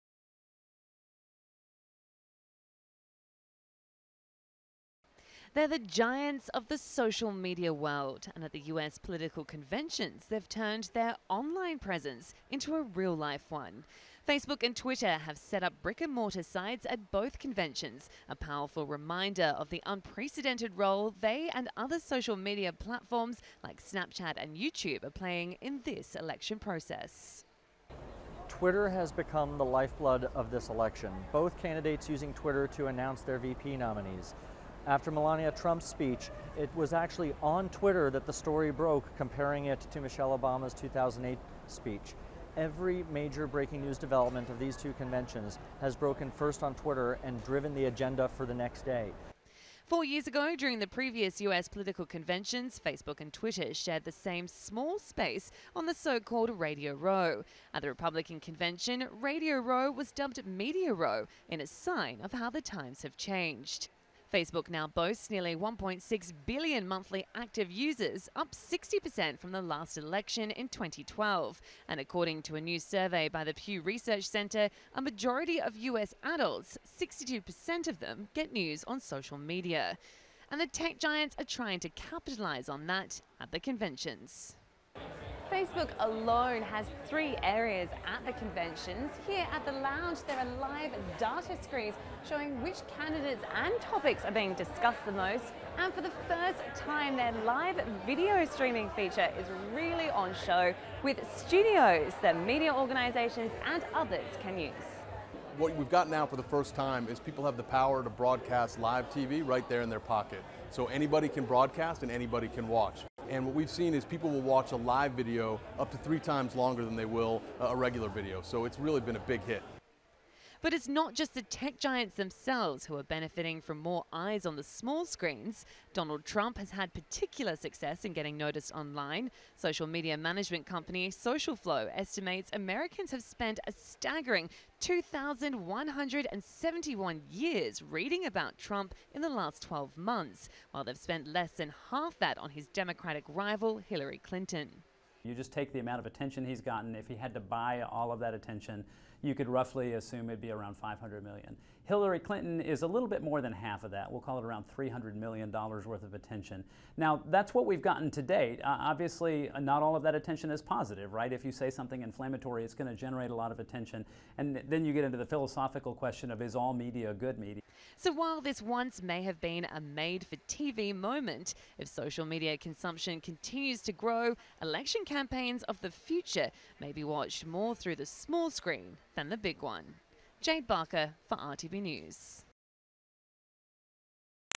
Social Media at the DNC
Reuters discusses the unprecedented impact of social media on the 2016 elections, and how the technology platforms and candidates are benefiting from that. Jim Anderson, CEO of SocialFlow shares insights on the attention that candidates, Trump and HRC have received on social media platforms. Watch the video to learn more!